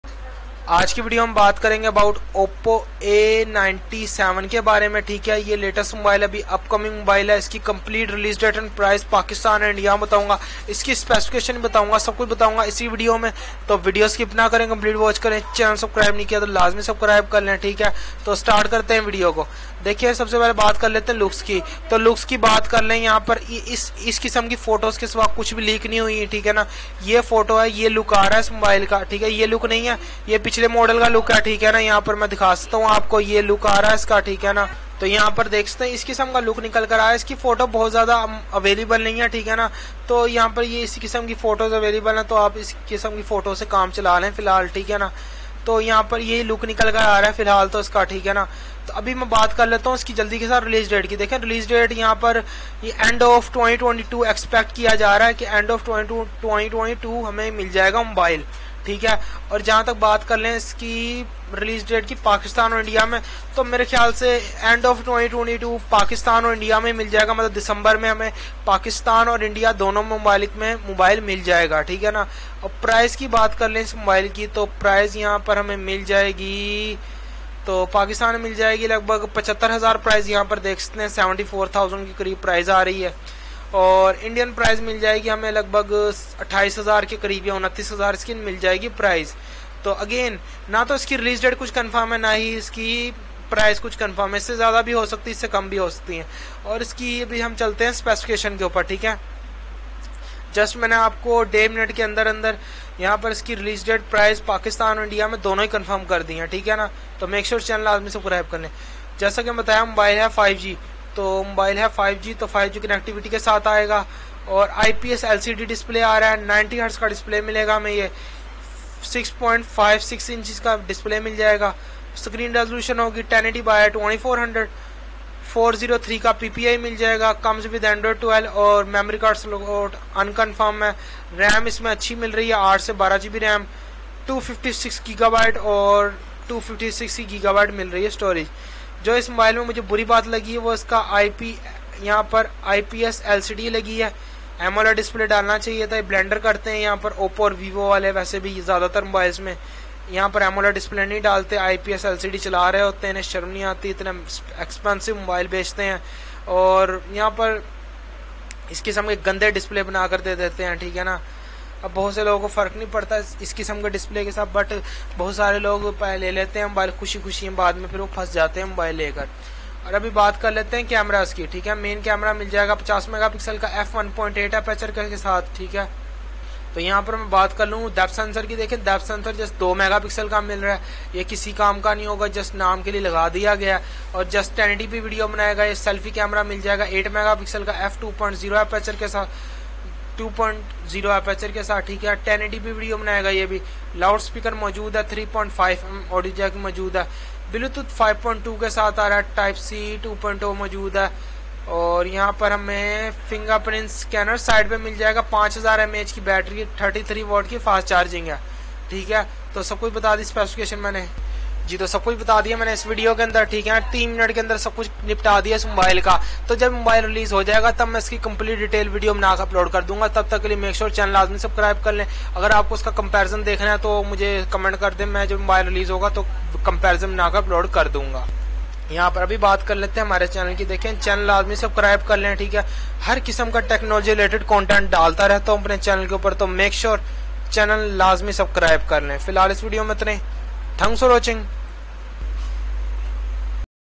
OPPO A97 5G Launch Date Price in Pakistan & India | OPPO A97 5G First Look , Review, full Spec
UsamaSaeedIT UsamaSaeed
Tech Videos  Upload Daily on YouTube in  Urdu Hindi

Topics covered in this video:

1.  OPPO A97 5G  Launch Date in Pakistan
2.  OPPO A97 5G  Launch Date in India
3.   OPPO A97 5G price in Pakistan
4. OPPO A97 5G  price in India
5. OPPO A97 5G  First Look, Review, Specification, Price, Setup, Camera